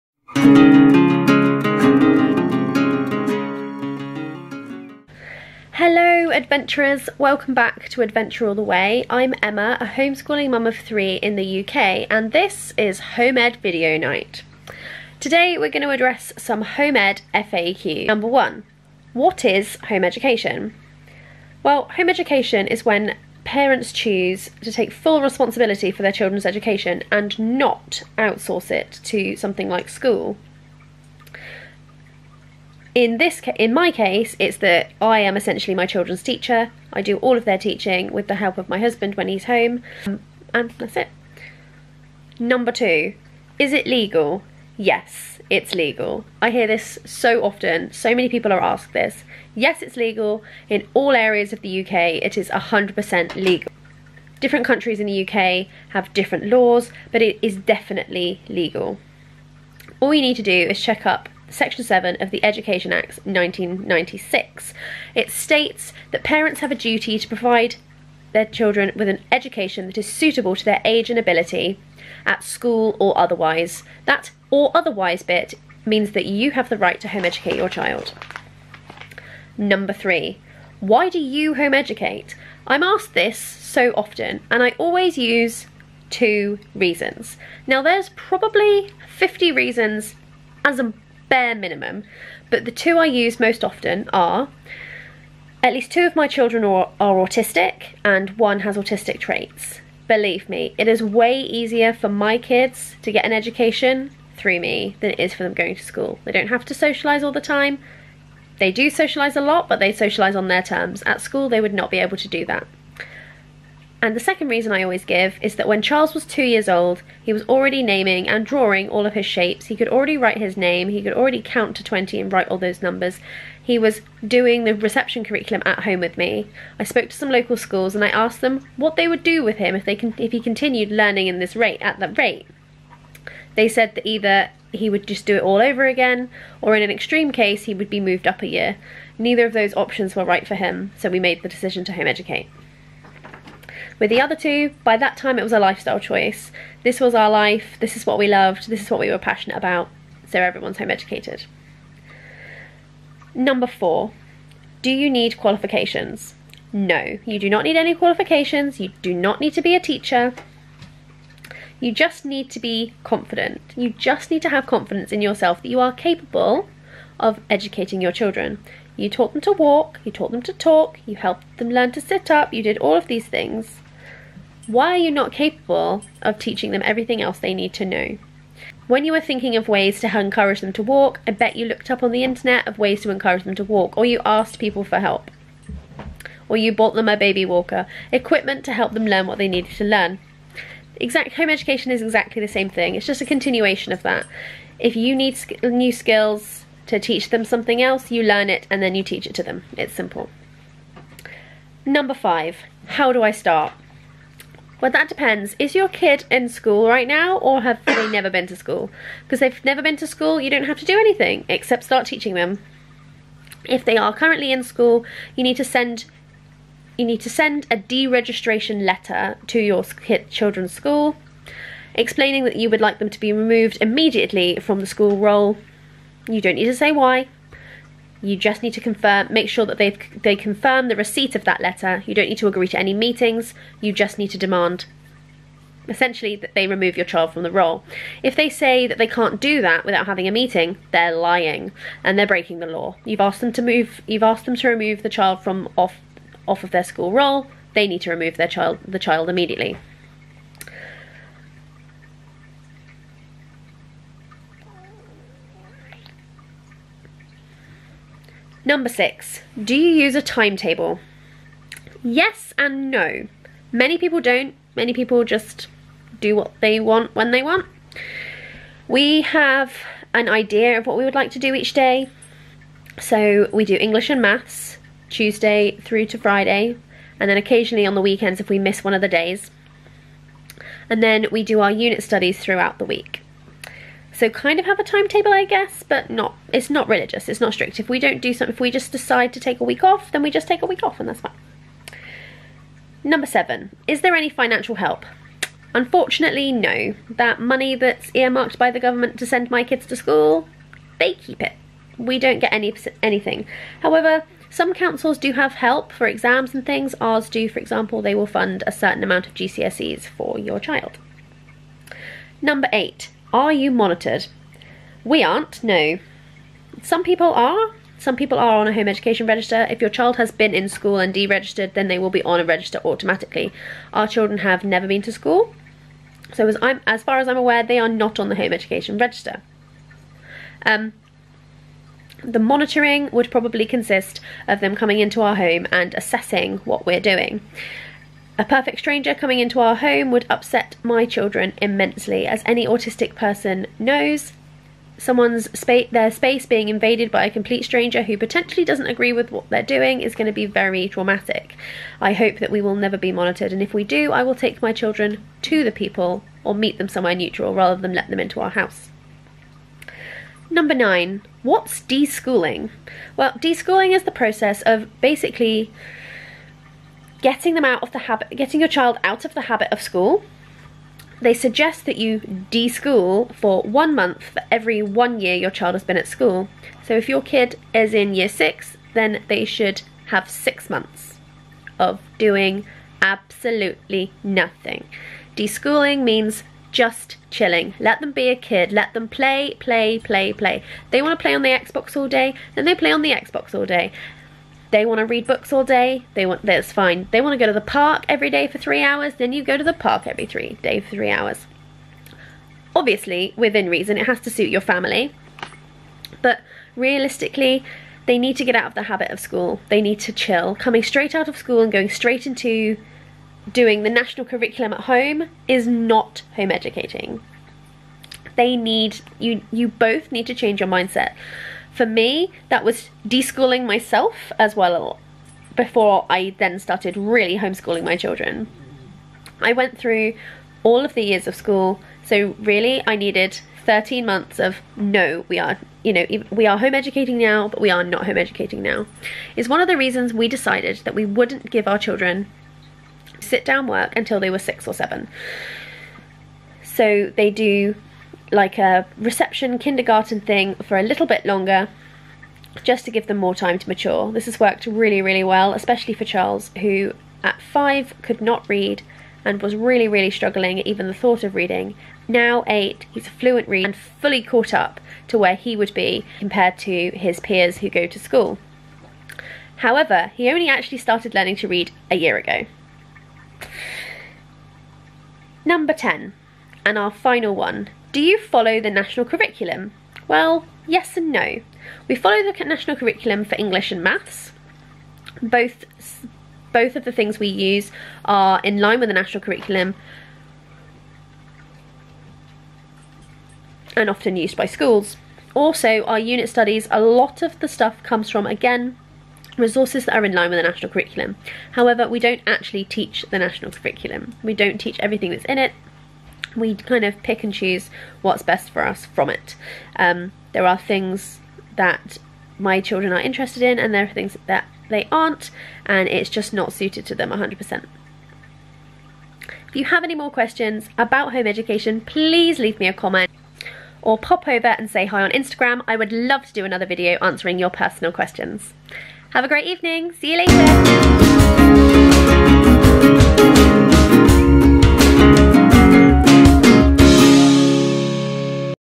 Home Education UK - Home Education FAQs - Homeschooling Mum of 3 answers common Home Education FAQs.
In this video I am answering 10 common home education frequently asked questions.
As a home educator or home schooler in the UK I am asked a lot of these questions all the time!